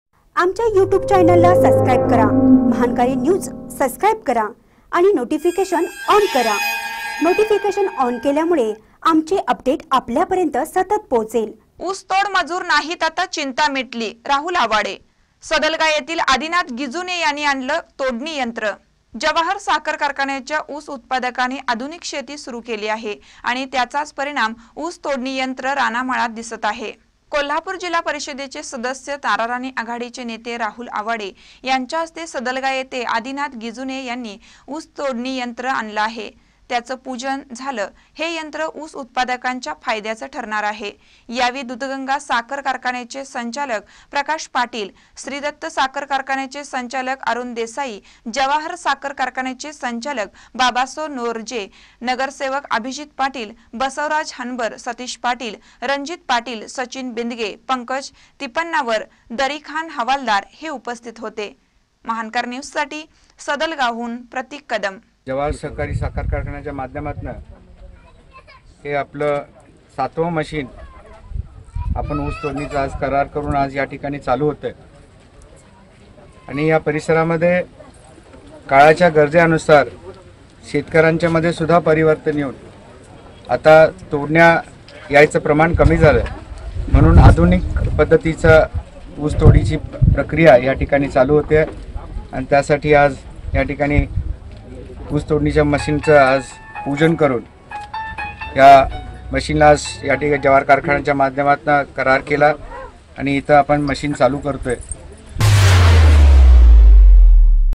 ऊस तोड मजूर नाहीत आता चिंता मिटली राहूल आवाडे
ऊस तोड मजूर नाहीत आता चिंता मिटली राहूल आवाडे सदलगा येथील आदिनाथ गिंजूने यांनी आणलं तोडणी यंत्र